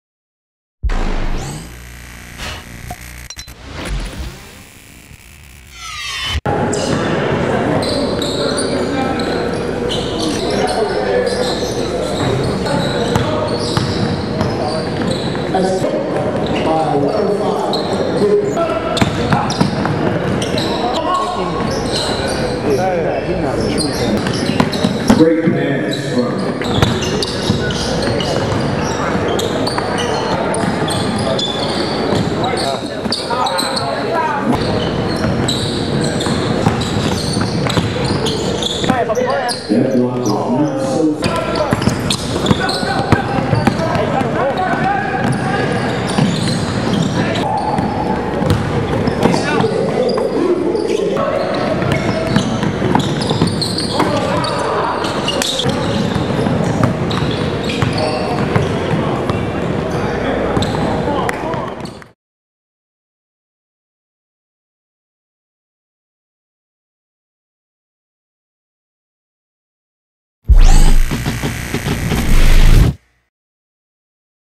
Marlon Hunter Class of 2015 Highlights from the 2012 Super Soph Camp
Marlon Hunter is a 6'2 prospect from the class of 2015.   Hunter attends Melrose High School in Memphis and runs with the Worldwide Renegades on the AAU circuit.  Hunter showed that he could play with elite competition at the 2012 Super Soph Camp in Atlanta this spring.